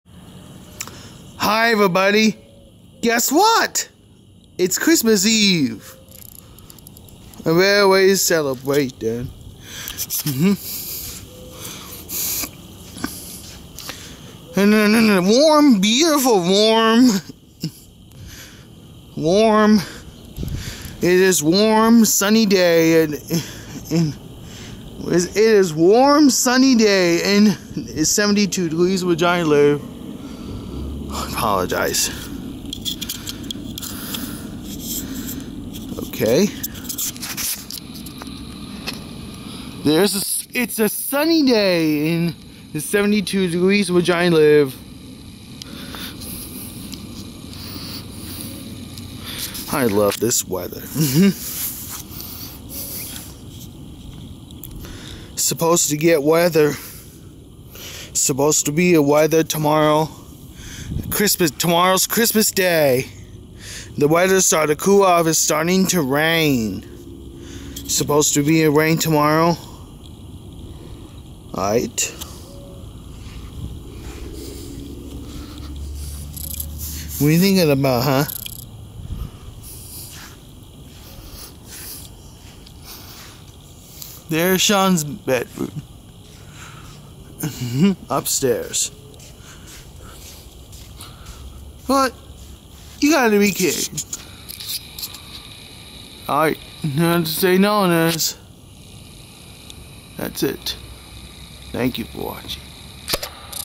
A Warm Sunny Christmas Eve Day - December 24, 2018
It is Warm Sunny Christmas Eve In Gilbert, AZ. It is Low 70s.